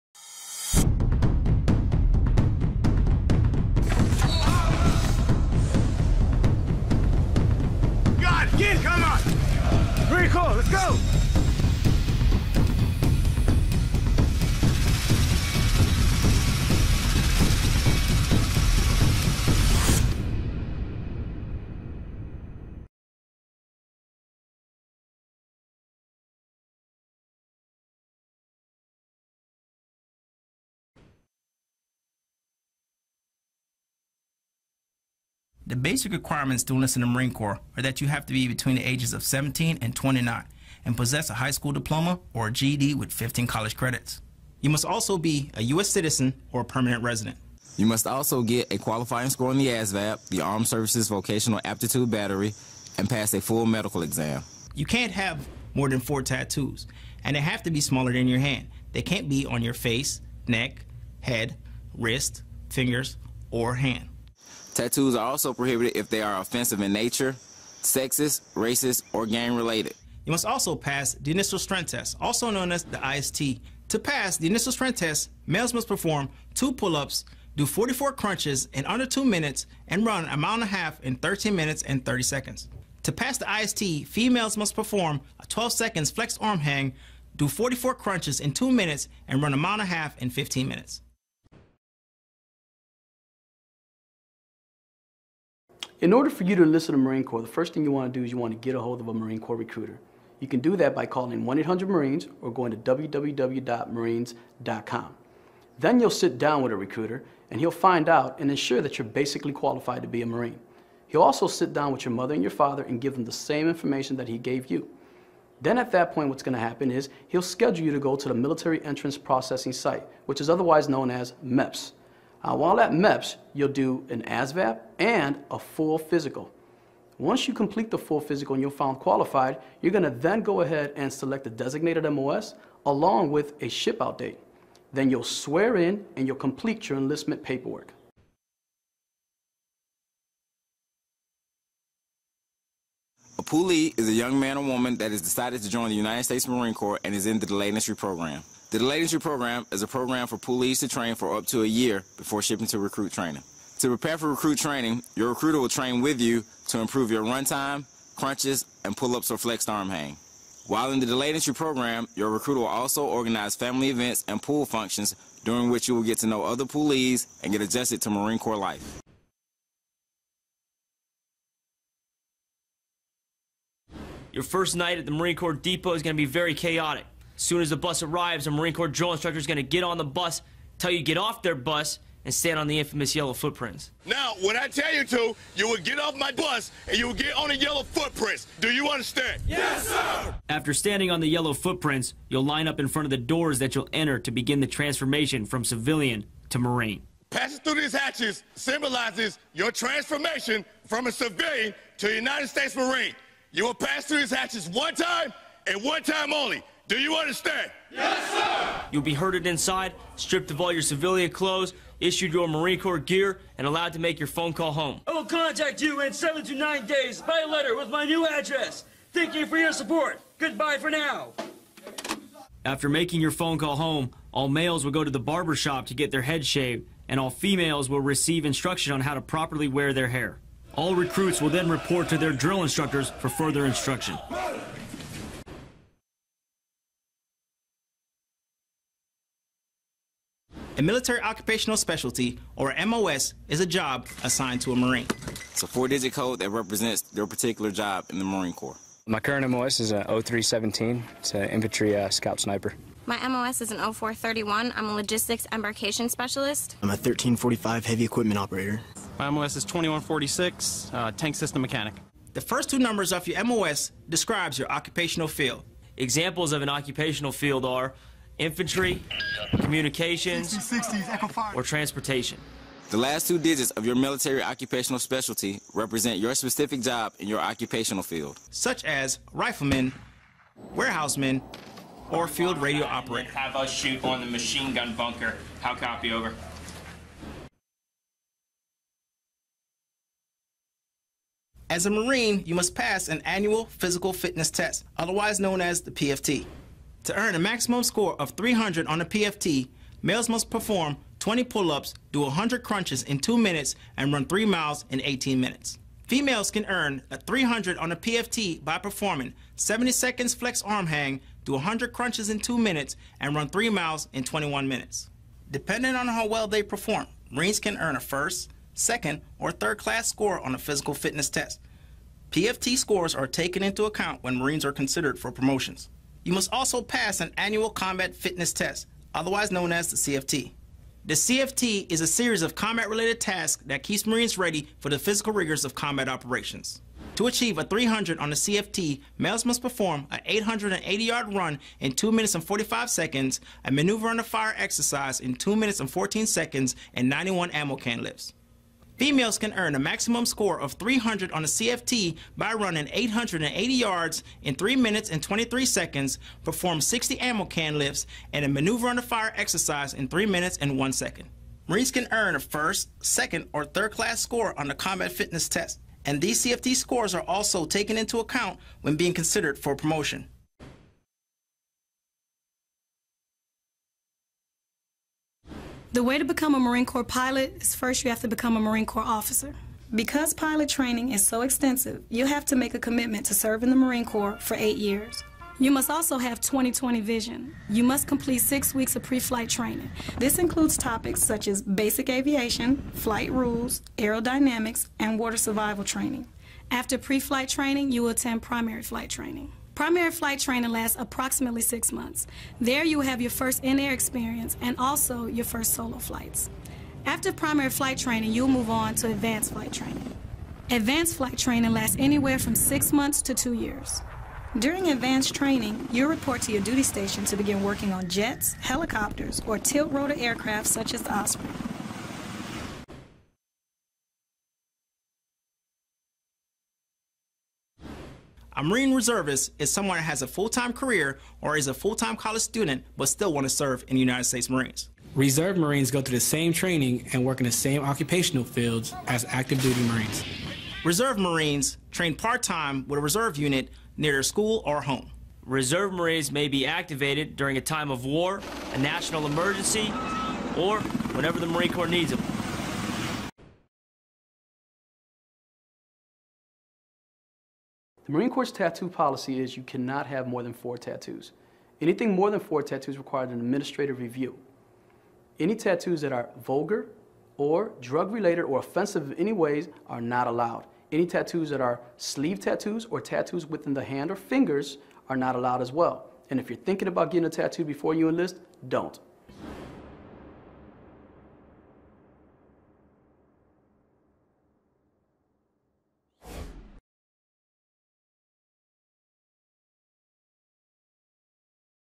Ask A Marine
The United States Marine Corps listens to the questions our community asks, and there's no one better to answer them than a Marine . . . And Marines do just that in this video by discussing the following...

1) Basic Requirements... Interested in age, education, tattoo, fitness, and other requirements that you must meet to become one of the Few and the Proud?

2) USMC Enlistment Process... Learn the basics of the Marine Corps enlistment process in the latest entry in our Ask A Marine series. To begin the journey to become a Marine, you've got to take the first, most important step.

3) Poolees In The DEP... Once you've enlisted in the United States Marine Corps, do you know what to expect as a poolee . . . the Delayed Entry Program (DEP)?

4) Arrival at Recruit Depot... Want to know what happens when you arrive for recruit training at Marine Corps Recruit Depot Parris Island or San Diego?

5) MOS...  Do you know what a MOS is? All Marines are assigned a Military Occupational Specialty, which is a 4-digit code that represents their job in the Marine Corps.

6) Physical Fitness... Do you know how the Marine Corps measures the physical fitness of Marines? Learn about the physical fitness test (PFT) and the combat fitness test (CFT).

7) Marine Corps Pilot - Do you know what it takes to become a Marine Corps pilot? Whether you want to fly an MV-22 Osprey or the F/A-18 Hornet.

8) Reserve Marines - Reserve Marines must go through the same 12 weeks of Recruit Training and must live up to the same standards as their active-duty counterparts — but do you know how they differ?

9) Tattoo Policy... Many Marine Corps prospects have questions about the Marine Corps tattoo policy -- now you can hear the answer straight from a Marine.

A compilation of U.S. Marine Corps videos - 2012

USA Patriotism!